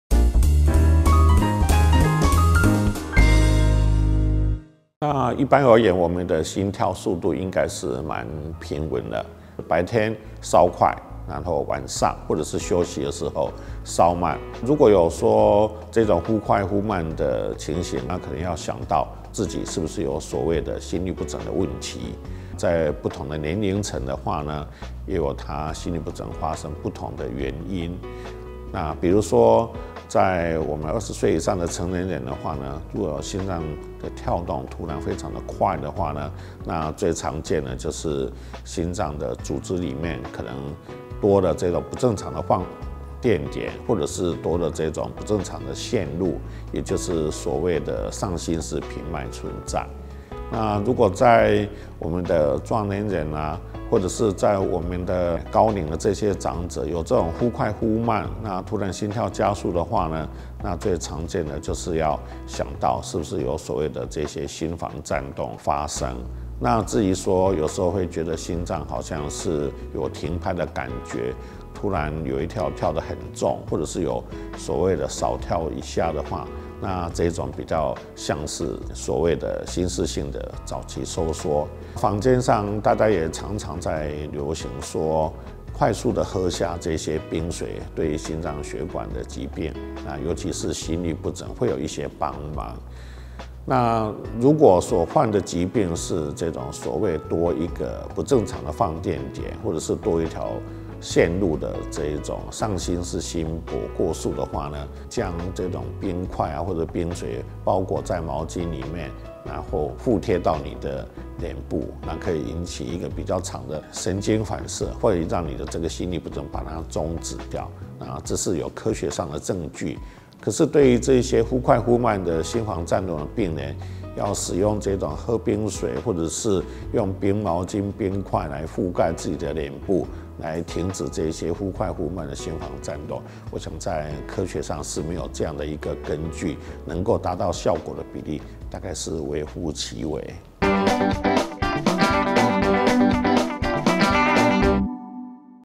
心跳忽快忽慢的3大原因！ 心律不整 喝冰水有效？【 名醫觀點 】 陳適安
《健康2.0》獨家聯名課程「貴妃浴面美容功」 搶購早鳥優惠價！